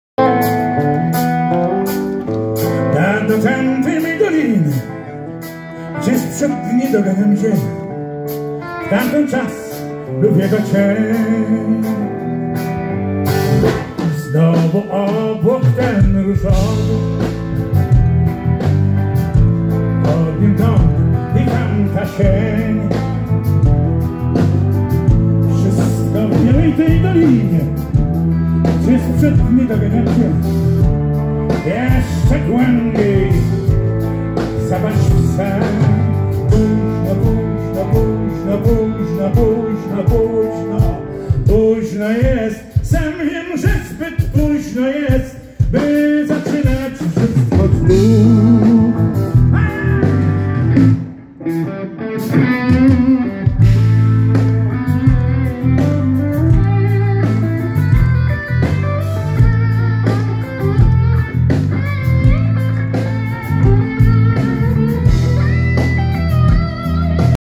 Krzysztof Cugowski - Średzkie Sejmiki Kultury 2023
Fragment koncertu Krzysztofa Cugowskiego zarejestrowany w Środzie Wlkp. 28 maja 2023 roku podczas Średzkich Sejmików Kultury